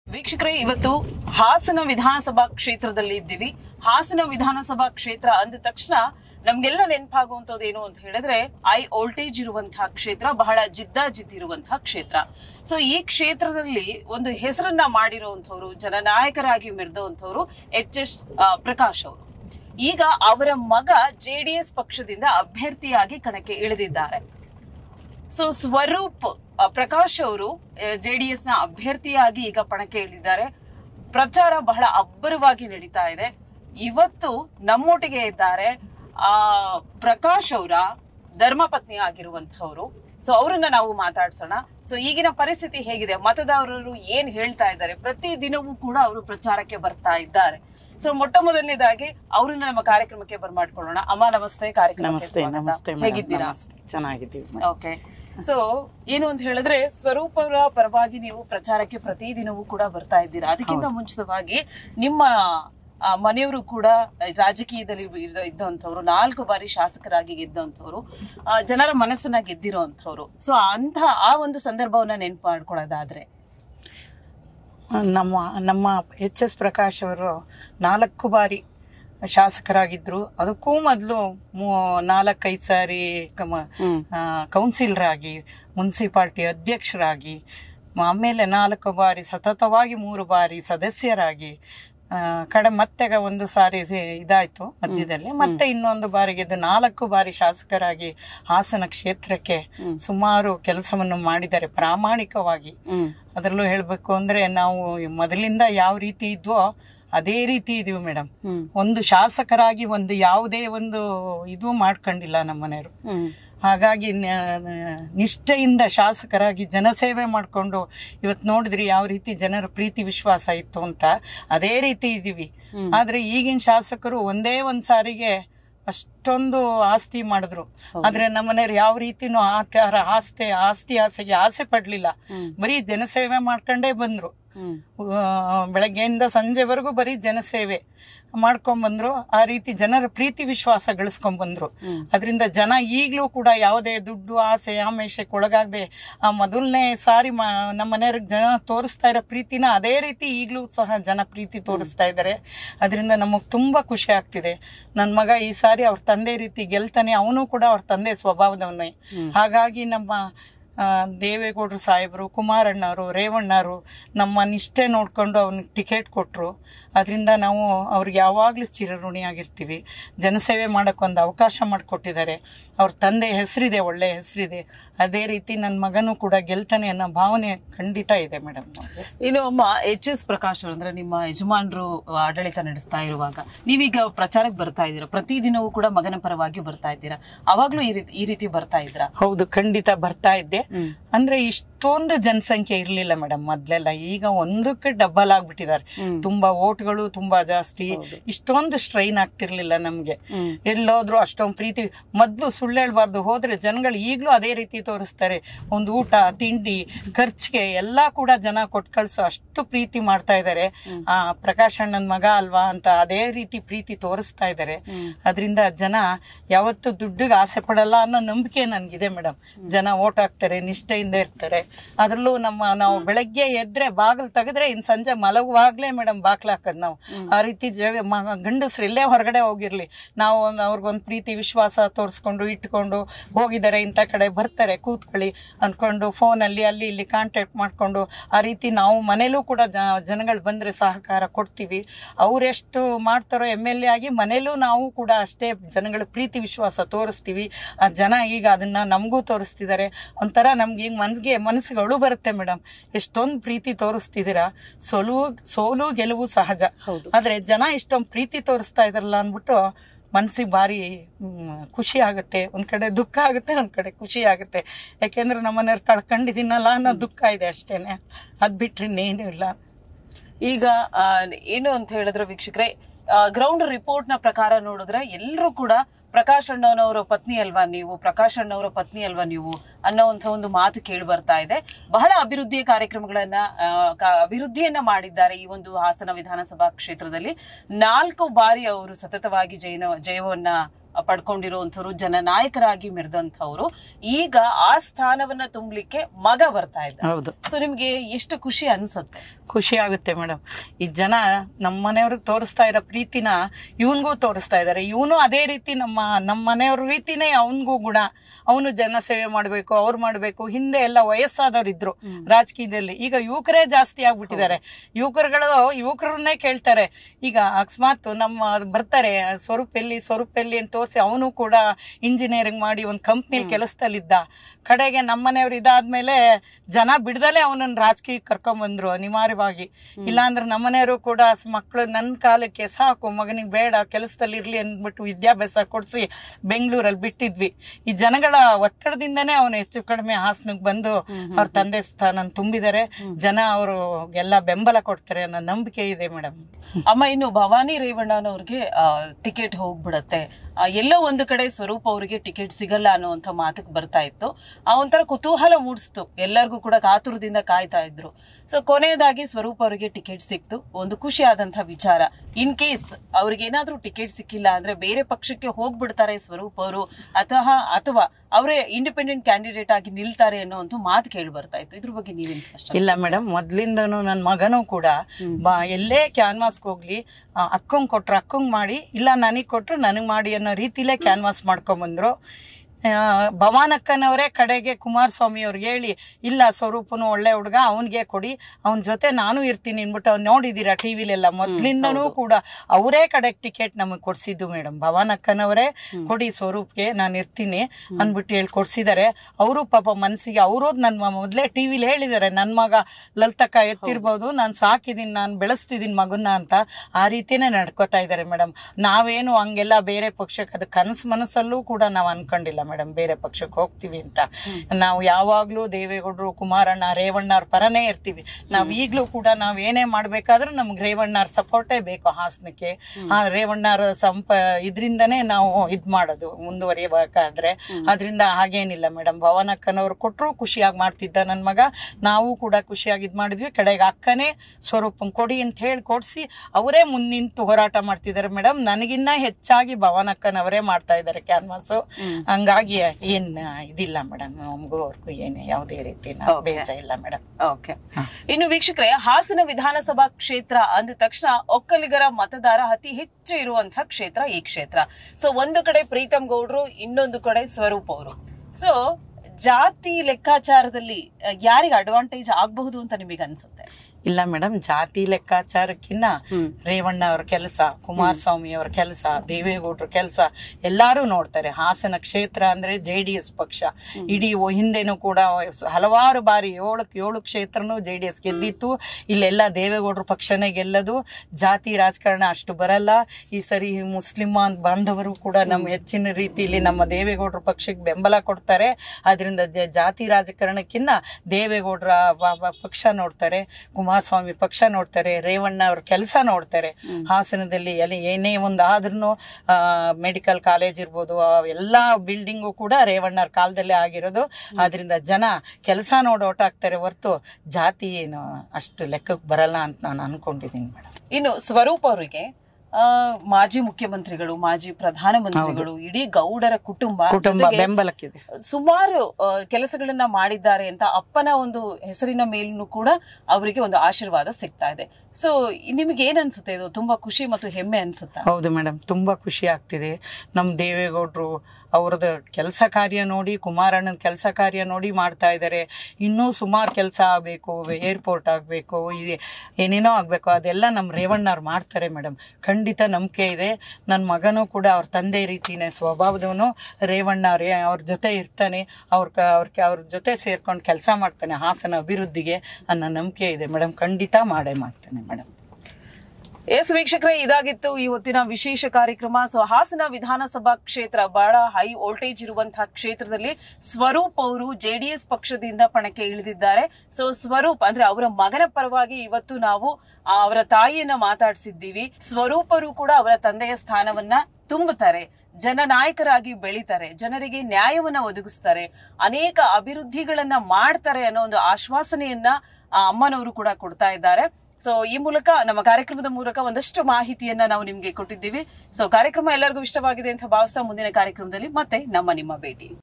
ಹಾಸನ ವಿಧಾನಸಭಾ ಕ್ಷೇತ್ರ ನನ್ನ ಮಗ ರಾಜಕೀಯಕ್ಕೆ ಬರುವುದು ಇಷ್ಟವಿರಲಿಲ್ಲ || Swaroop Prakash || EesanjeNews.
ಹಾಸನ ವಿಧಾನಸಭಾ ಕ್ಷೇತ್ರ ನನ್ನ ಮಗ ರಾಜಕೀಯಕ್ಕೆ ಬರುವುದು ಇಷ್ಟವಿರಲಿಲ್ಲ | ಹೆಚ್.ಎಸ್.ಪ್ರಕಾಶ್ ಅವರ ಪತ್ನಿ | Swaroop Prakash

No.1 Kannada Evening Newspaper Published From Karnataka